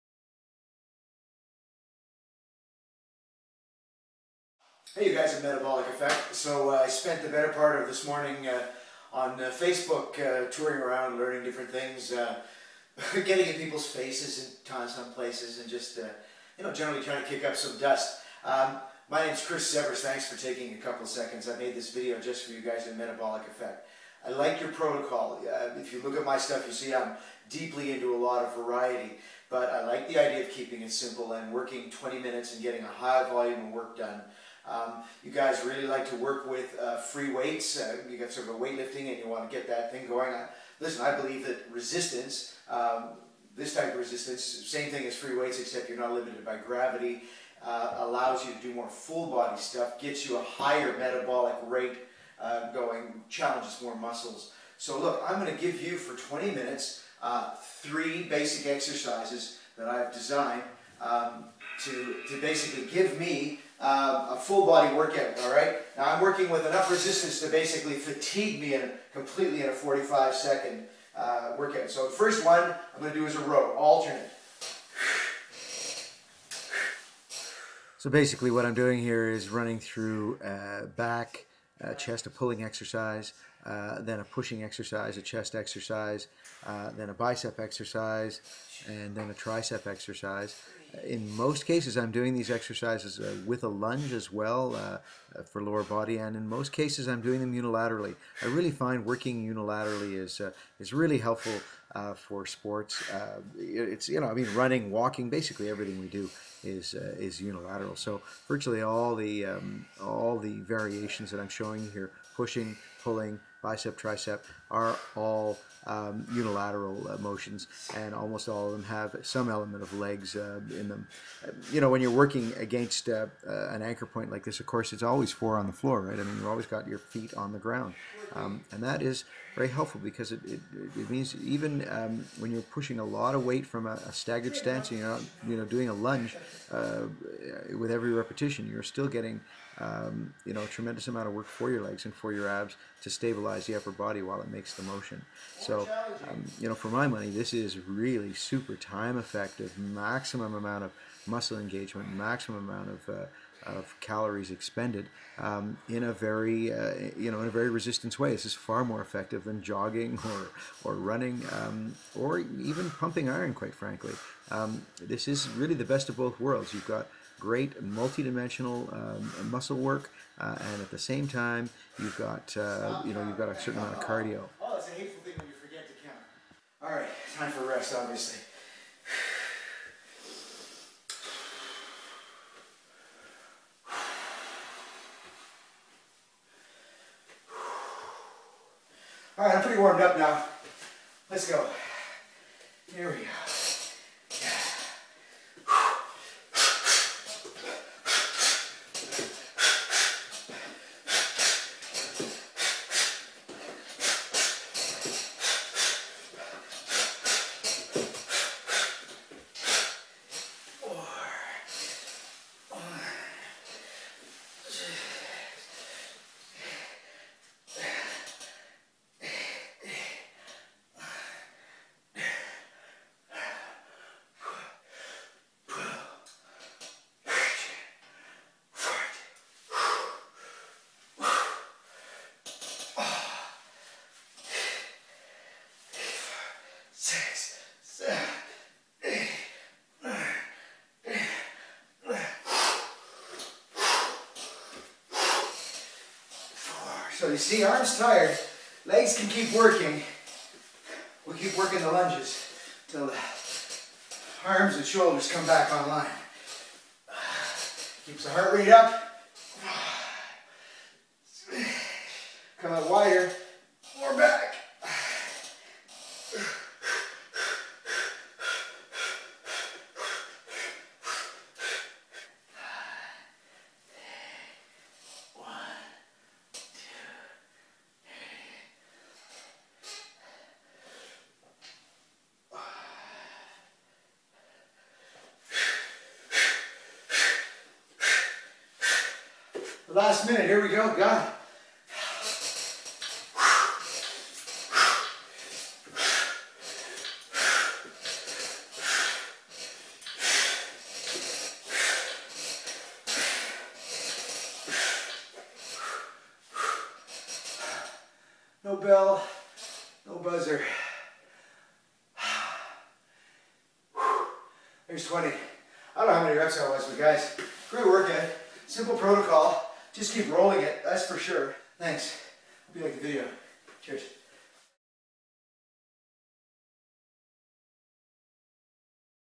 Full body metabolic workout - 20 minutes
Here are three basic exercises done eight reps each side, over 20 minutes, total of 11 sets